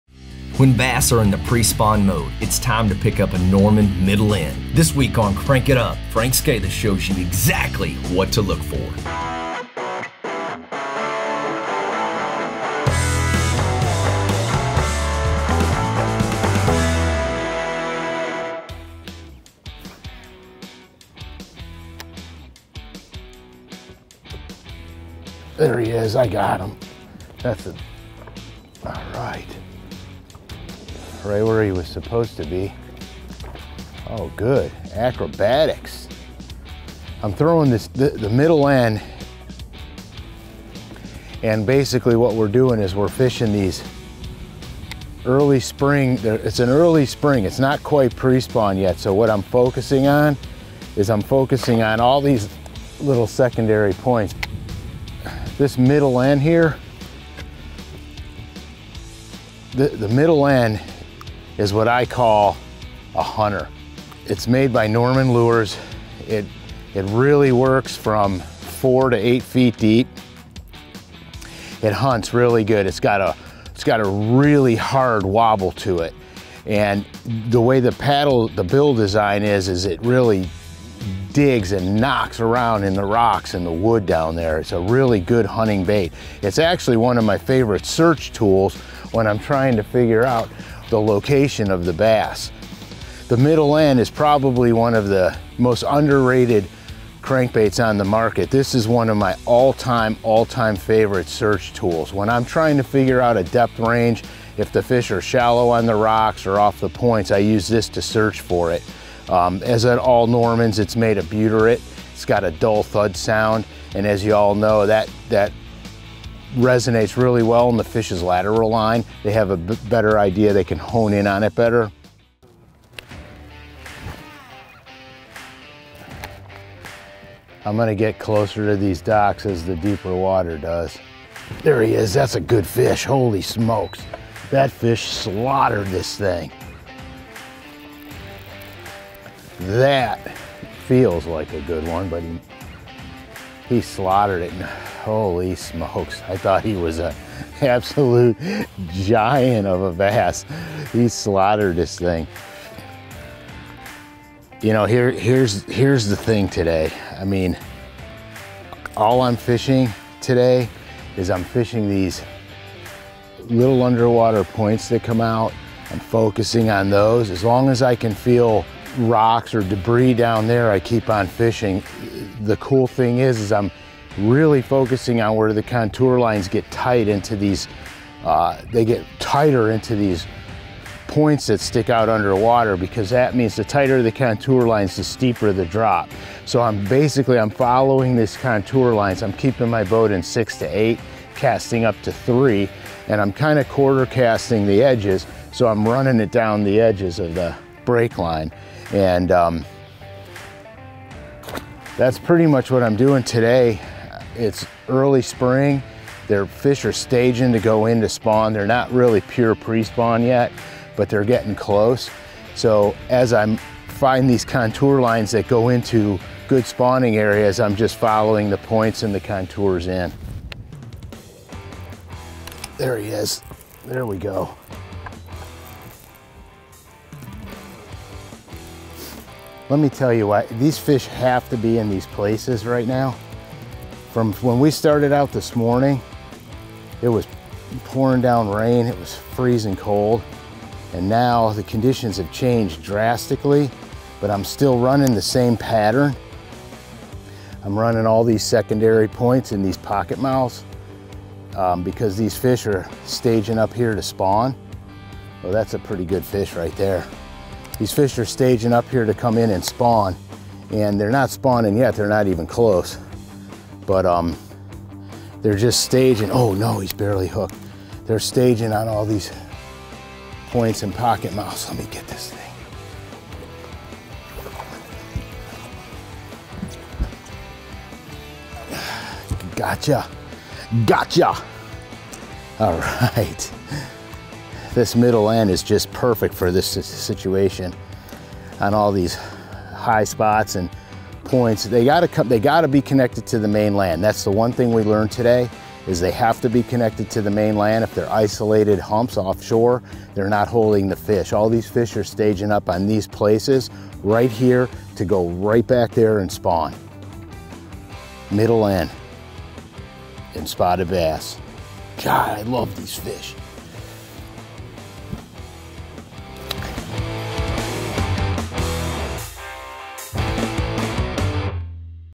Top Early Spring Crankbait Patterns for Bass – Crank It Up
Learn why crankbaits are so effective for early spring bass.  Watch as Frank Scalish un-locks one of his top patterns for catching early spring spotted bass.